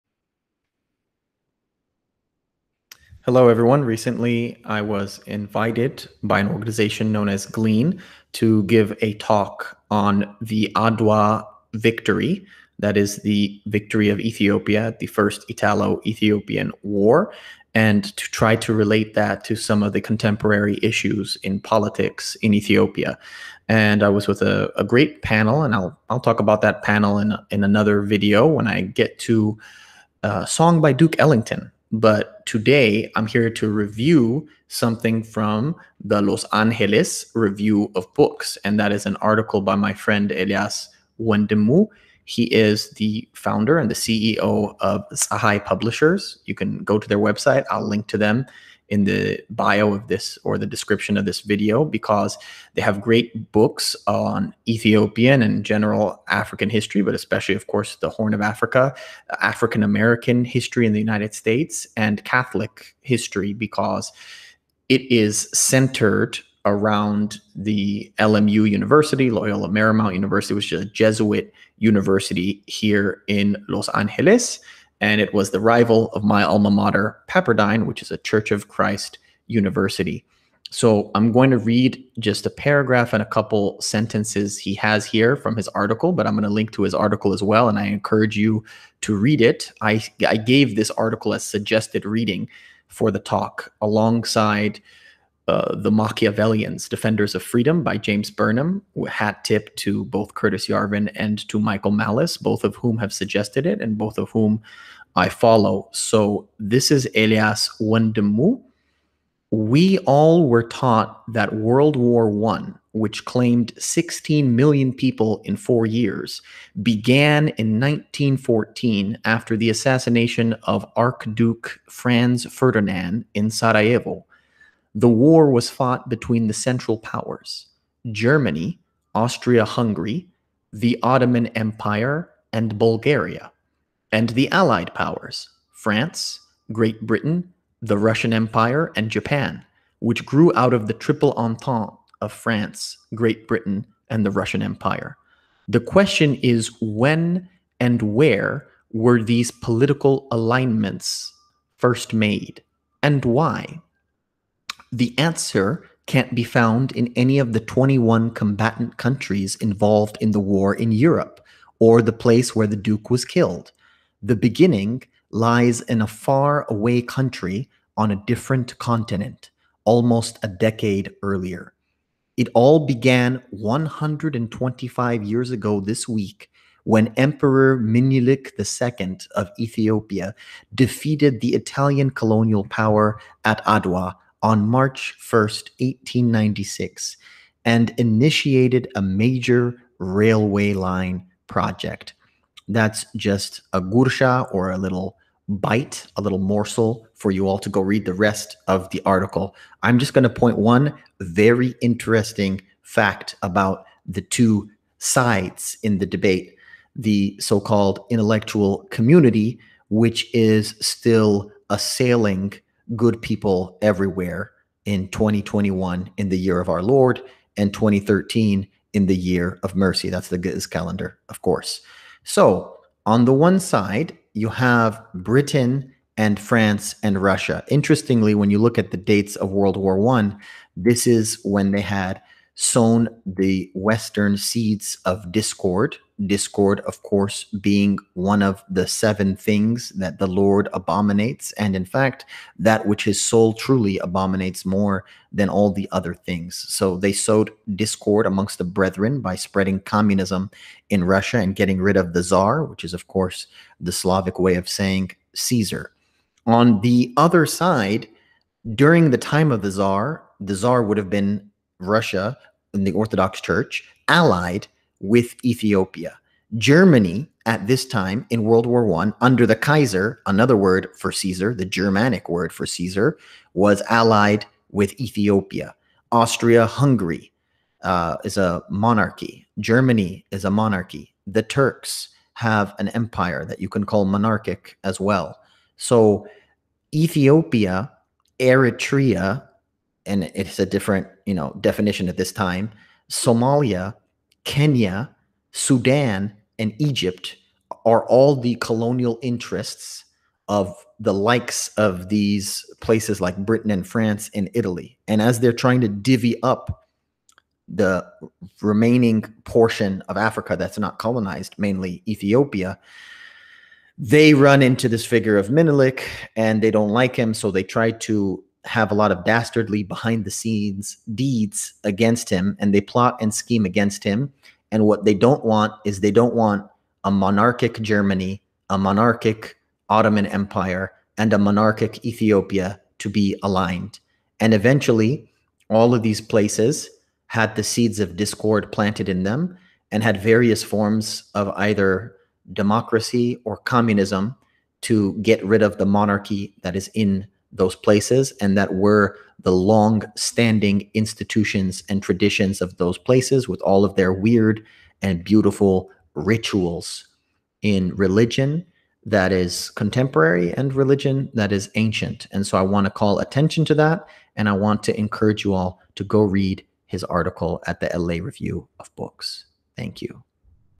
Review: Ethiopia: Where 2 World Wars Began & Where the 3rd Is Brewing
In the LA Review of Books, Tsehai Publisher's Elias Wendimu says that the US regime better tread carefully when approaching the current situation in Ethiopia. There are echoes of yesteryear. And yesteryear wasn't the seventh heaven. He says China is a vast nation-state to watch out for. I'd add Russia to the list. Go read it. And buy his books.